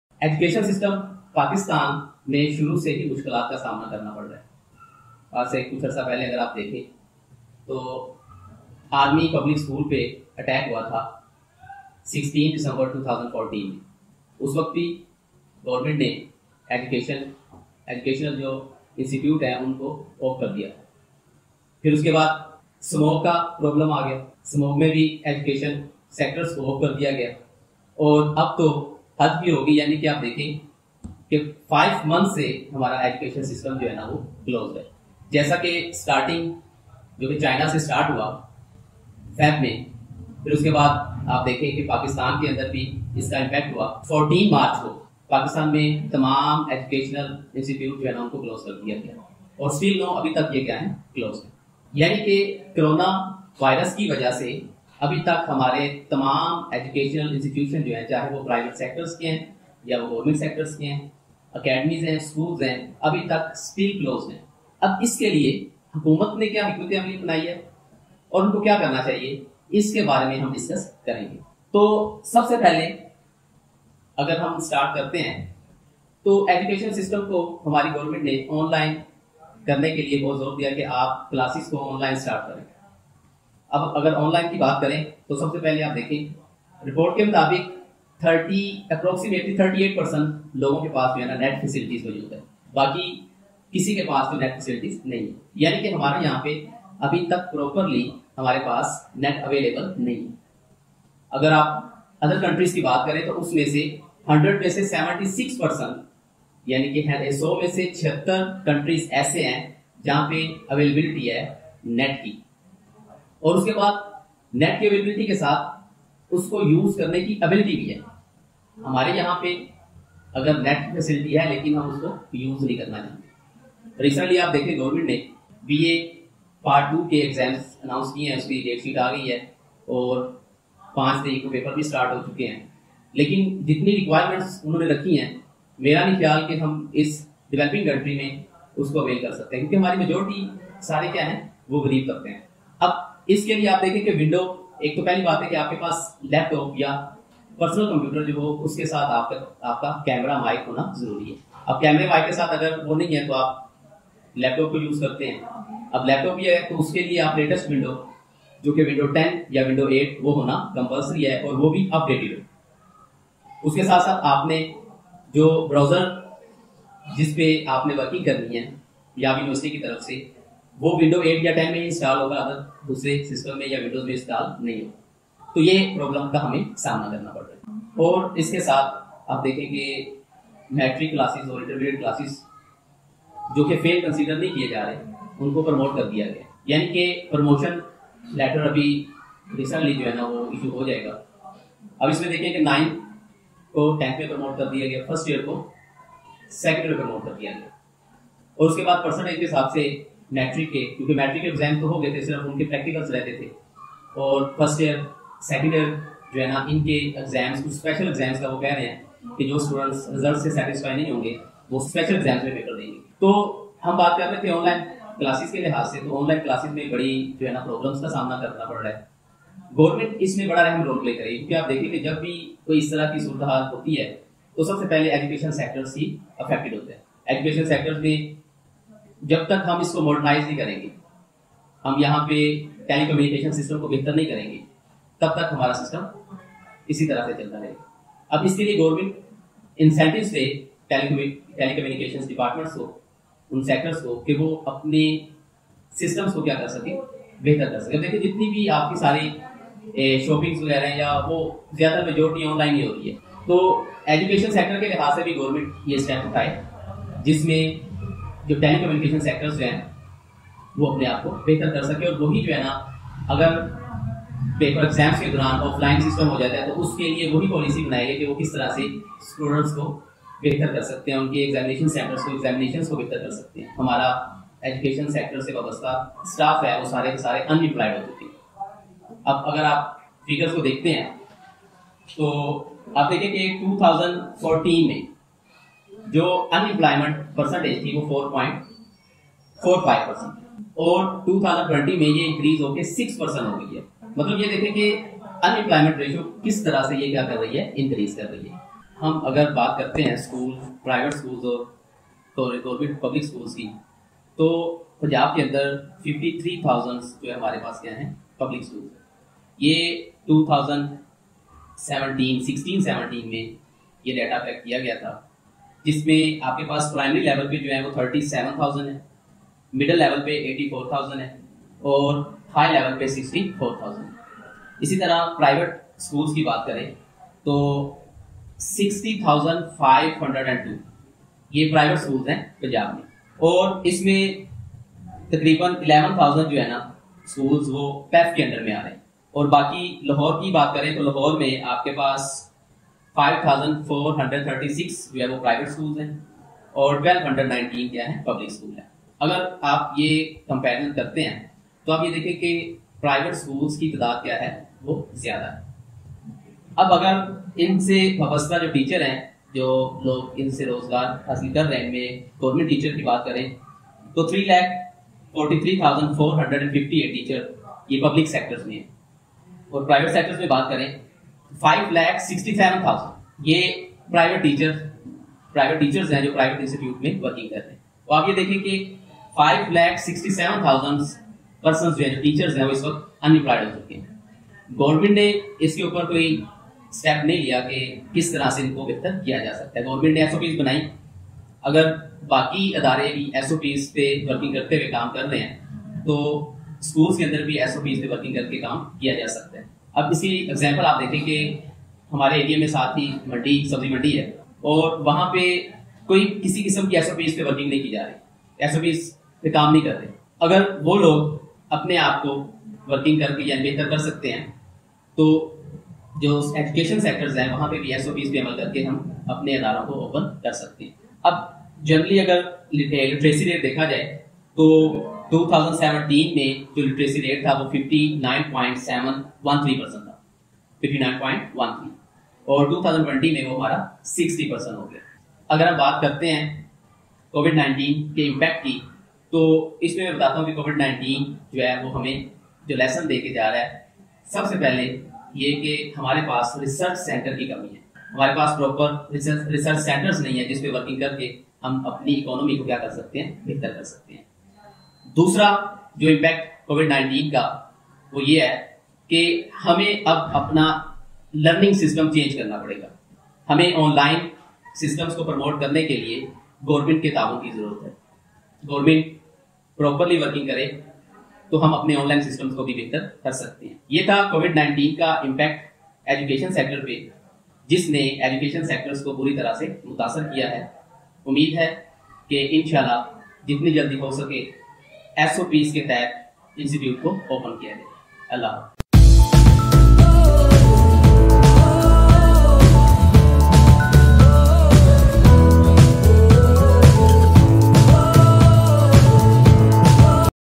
Impact of covid 19 on education sector of Pakistan
Salam, In this video Prof. Nawazish Ali tells us about the effect of COVID-19 on education sector of Pakistan. Furthermore, he collected some facts and figures about education sector like no. of private schools and public schools in Punjab, before COVID-19 literacy rate and after COVID-19 literacy rate, total teaching staff in private schools and public schools etc.
This video takes your 8 minutes approximately. Keep watching till end and don't forget to give us your precious feedback. Jazakallah.

Regards: Team of Economics Portal